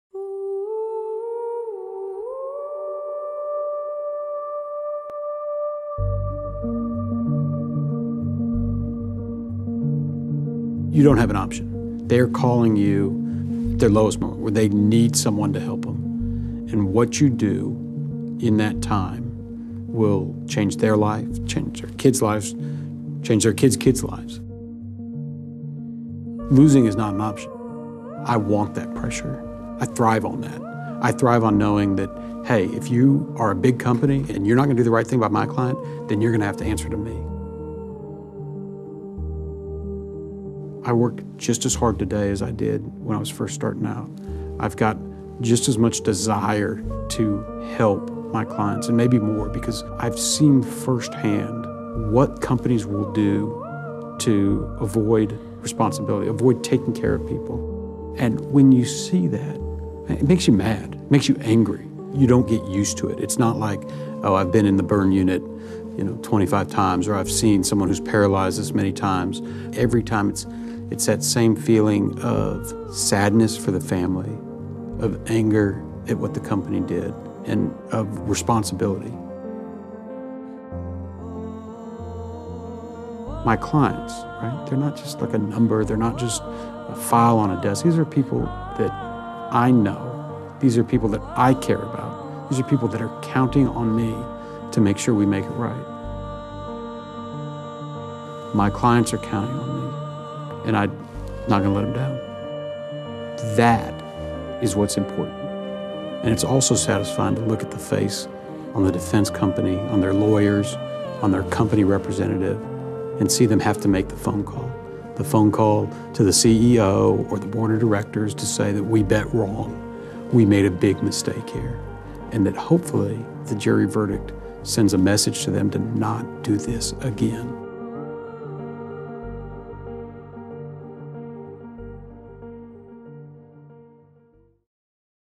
Meet Jason Itkin
Many people consider Jason Itkin to be the country’s top trial lawyer.

Since co-founding Arnold & Itkin in 2004, he has recovered billions of dollars for his clients. He’s one of only four lawyers in the country who’s won a billion-dollar verdict for a single person. He’s also won countless multi-million-dollar victories in courtrooms across the country—having represented clients in all 50 states.

Jason is known from coast-to-coast for his unyielding commitment to justice and his passion for helping victims rebuild their lives.

In every case Jason handles, he’s all in.

When bad things happen. When someone is in the fight for their life.

That’s when Jason shows up.

Whether the accident happens on an oil rig, a chemical plant, or a highway, people call Jason when everything is at risk. People know he can help. No matter the obstacles in his way, Jason will find a way to win for his clients.

Watch this video to learn more about Jason.